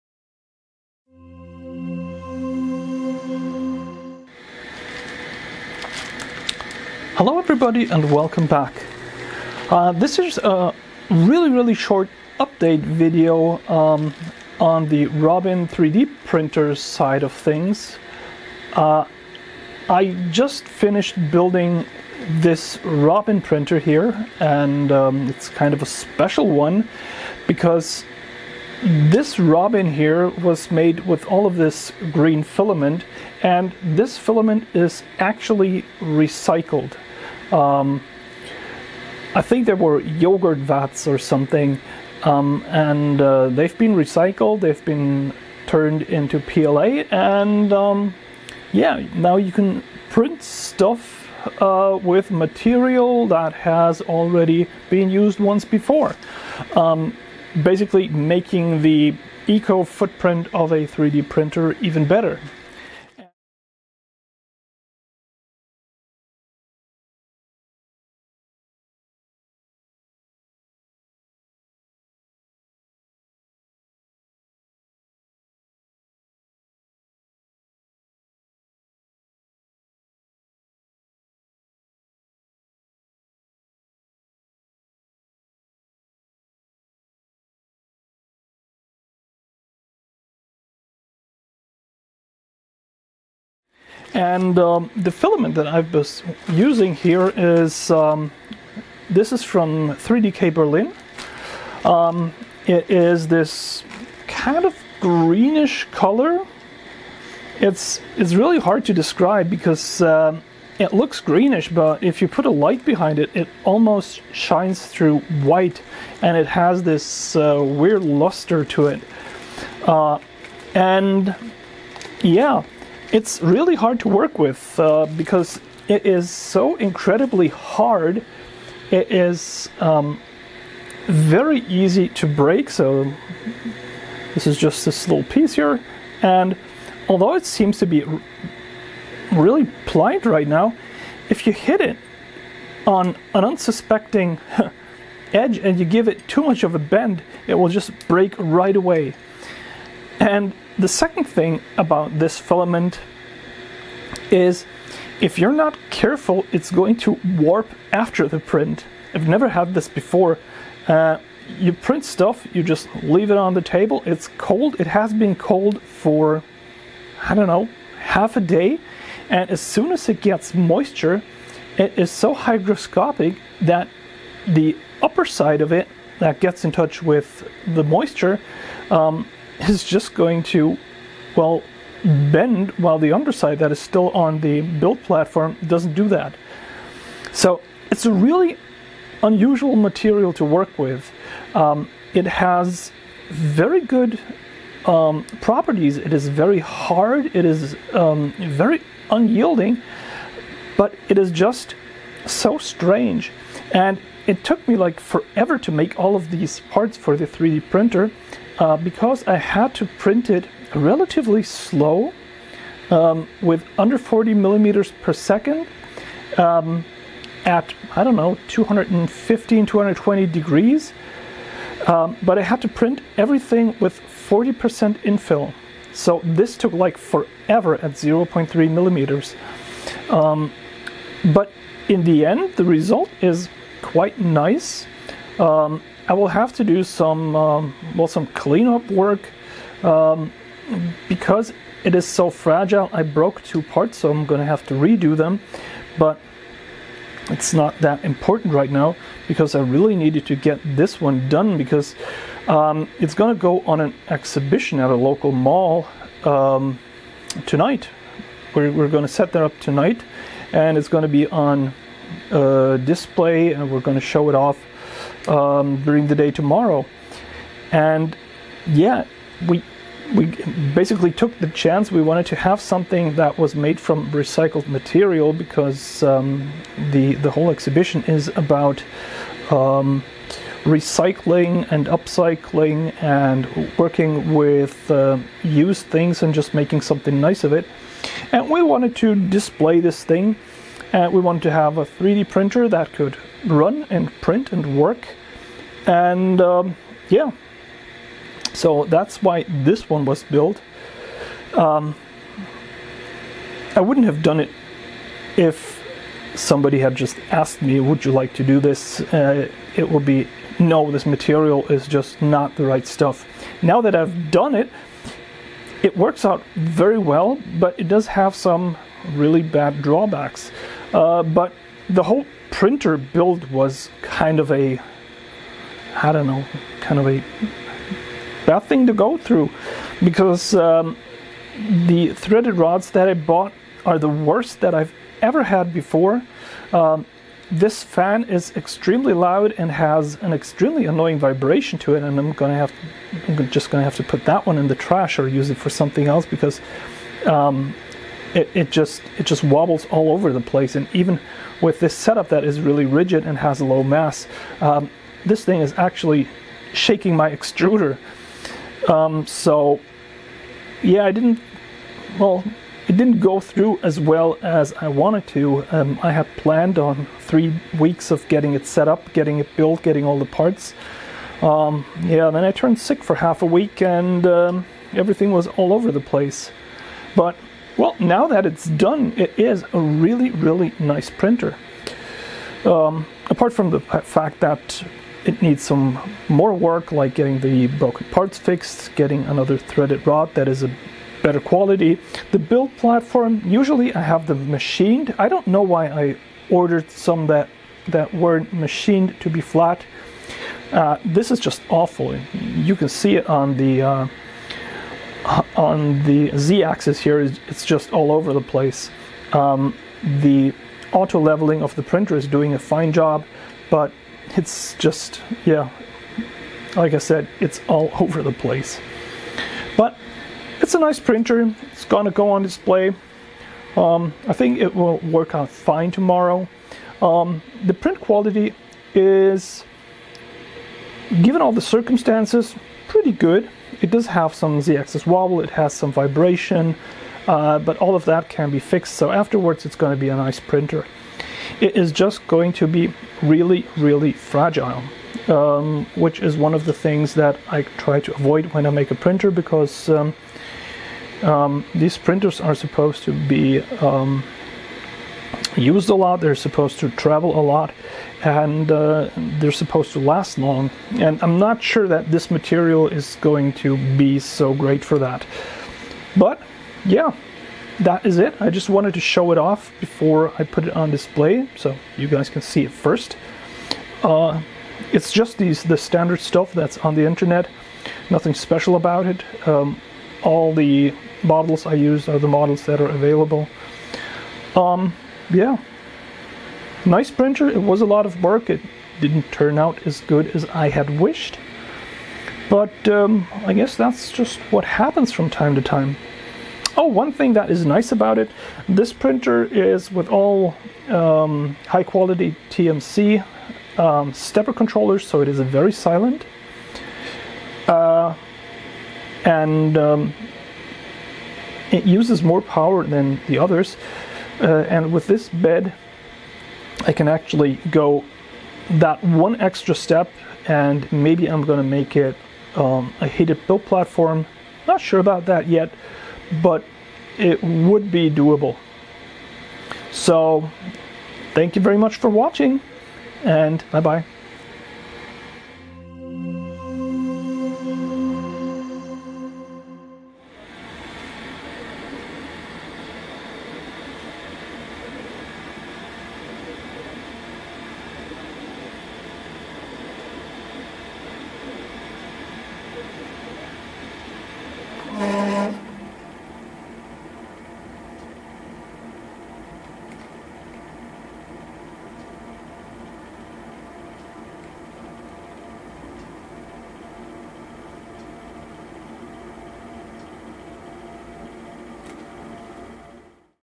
The Eco-Friendly Bad Robin 3D Printer (one-of POC)
I built one Bad Robin 3D printer with all the 3D-printed parts made of a recycled plastic based PLA filament.
This printer is a one-of for a local exhibition/show-n-tell event happening tomorrow. The key feature and reason was to demonstrate that you can use the recycled 3D-print filament to build key parts of a machine, and not just use it as a filler where physical properties don't count.

The filament I used is from 3DK Berlin, and according to the documentation available it is made from recycled yogurt and pudding containers (PP).

This filament has unusual properties for PLA, being very hard and brittle and is prone to warping while and after printing due to its very hygroscopic nature. That being said - successful prints for machines do work very well.

I do not recommend using this filament for every-day prints, it's a pain to work with, and needs very special printing profiles and care.

If you like what you see, and want to help out making this sort of development and production possible in the future, please consider a small donation at:
This helps a lot in keeping the channel and its content free and un-influenced.